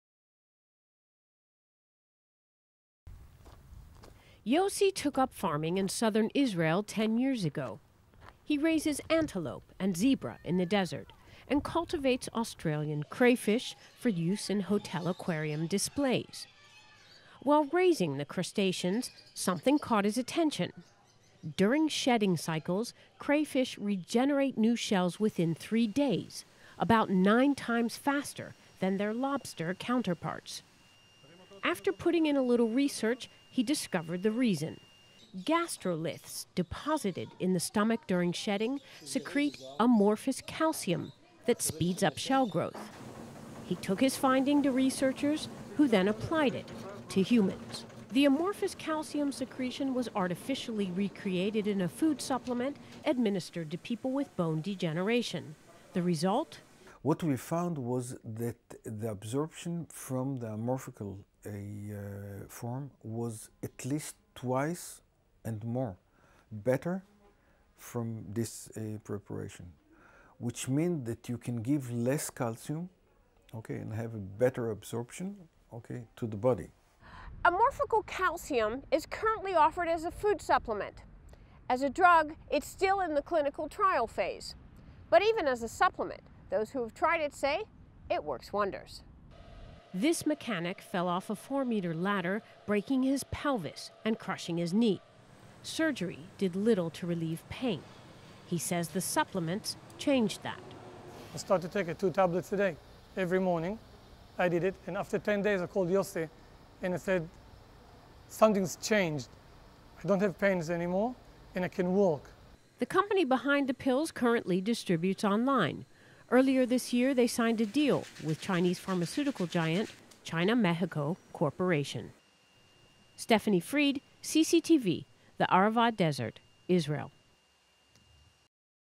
Osteoporosis: The Surprising Solution
.The Blue Crayfish is the inspiration for the development of an Israeli farmer named Yossi Ben
The Amorphous Calcium named Density is helped a lot of people to increase Bone Density and increase Osteoporosis